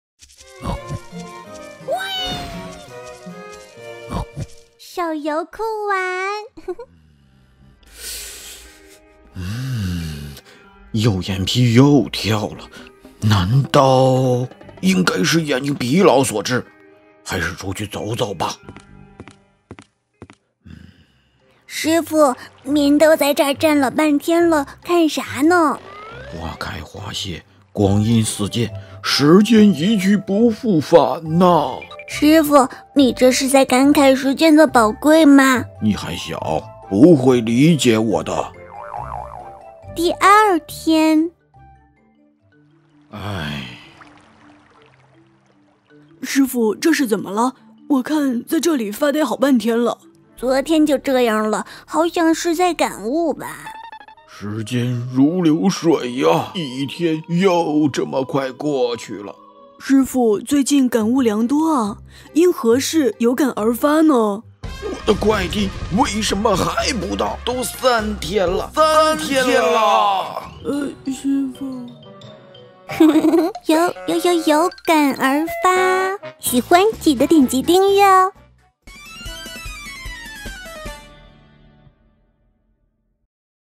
【植物大战僵尸同人动画】有感而发
最近气功僵尸有很多关于时间的感悟，武僧僵尸也想请教一下，他能否学到些什么呢？让我们一起来看看动画吧。